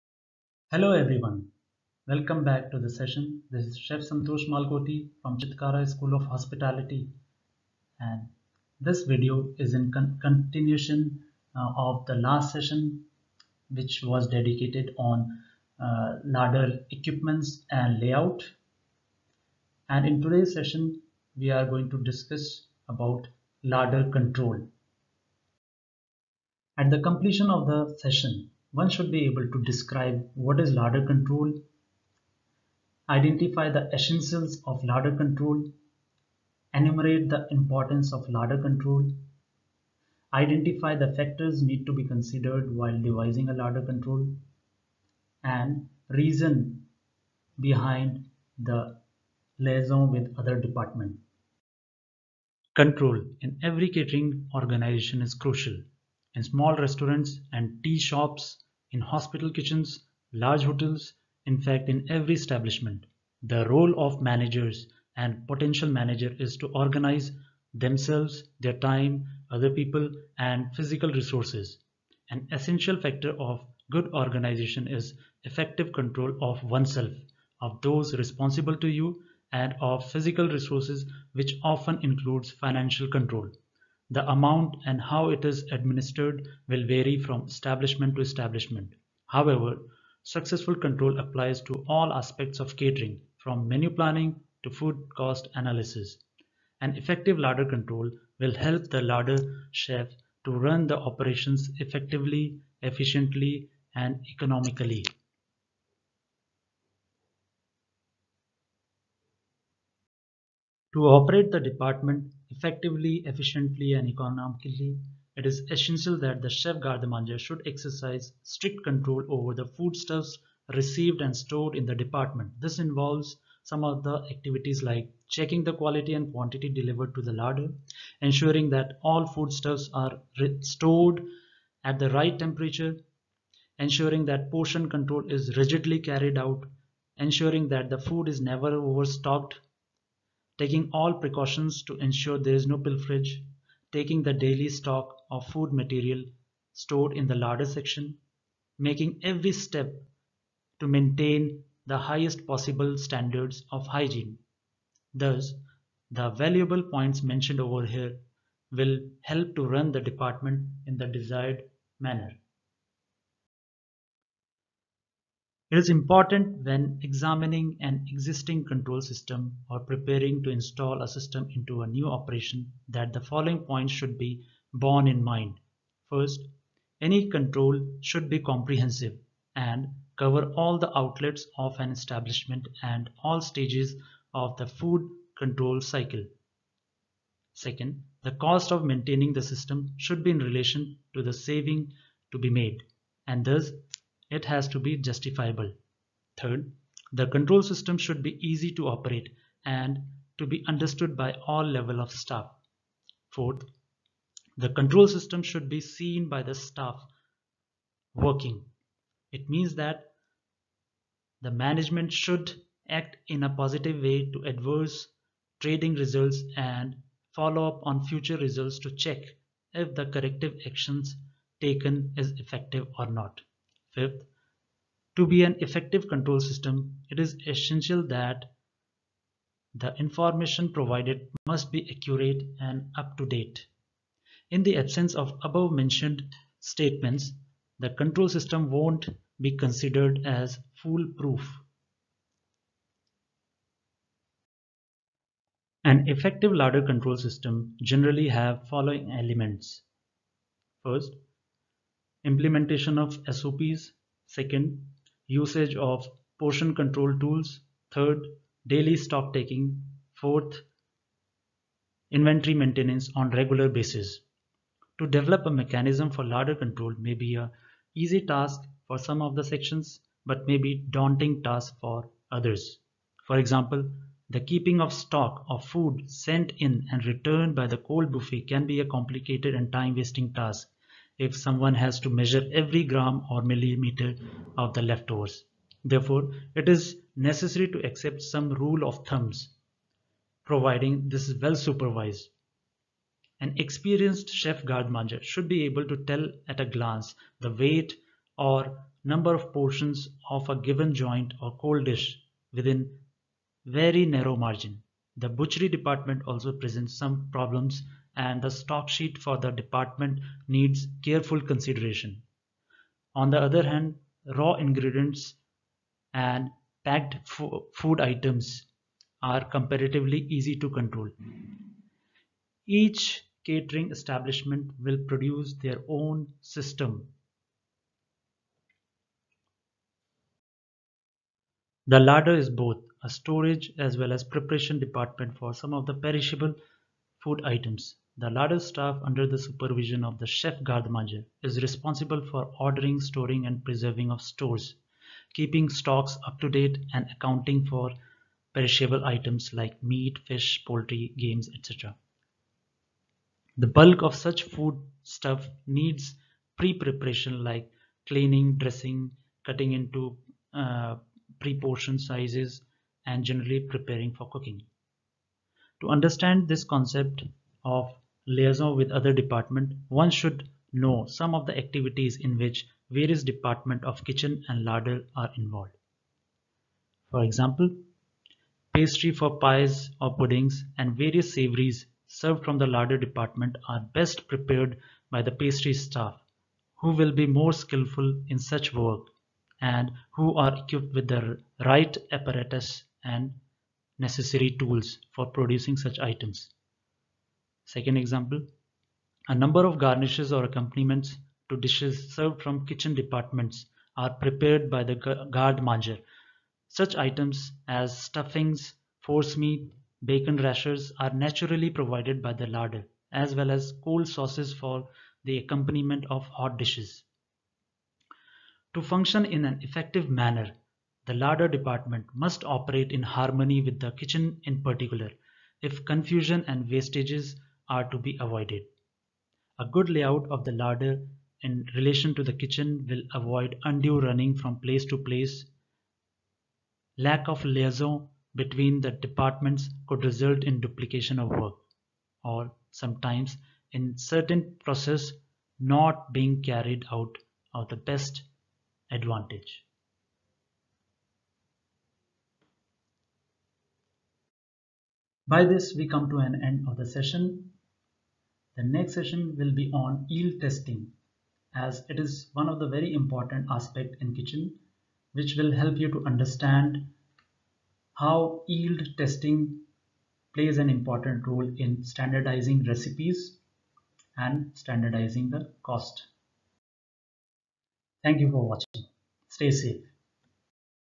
Larder Control
Control in every catering organisation is crucial: in small restaurants and tea shops, in hospital kitchens, large hotels, in fact in every establishment.

Essentials of Larder Control
To operate the department effectively, efficiently and economically, it is essential that the chef garde manger should exercise strict control over the foodstuffs received and stored in the department.

Devising Larder Control System
For some sections, the devising of a simple but effective list is reasonably easy. With others, it is not quite as easy. For example, the keeping of the stock of food sent in and returned by the Cold Buffet can be complicated and time-wasting, if one has to measure every gram or millimetre.